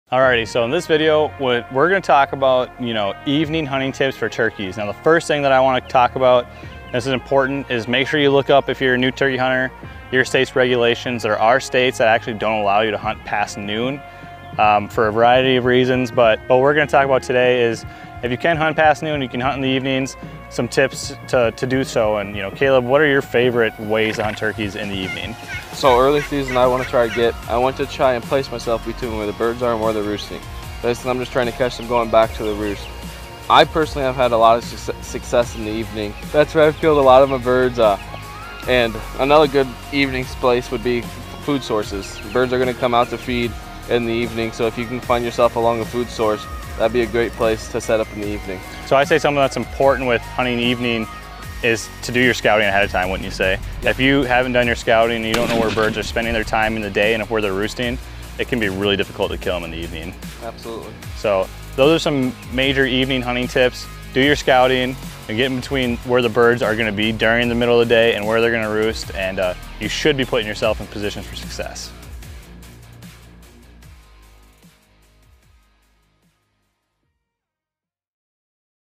At the Ranch - Turkey: Evening Hunting Tips
The first thing you need to look at with evening hunting tips is to make sure it’s legal in your state. Some states don’t allow turkey hunting after 12:00pm. The biggest tip with evening hunting is to know where birds are roosting and where they are spending their time in the middle of the day. If you know this, you’ll want to setup between the two areas to catch birds as they head back to roost. Food sources can also be a great evening setup. Scouting ahead of time will be the best way to figure out where you should be setting up in the evening.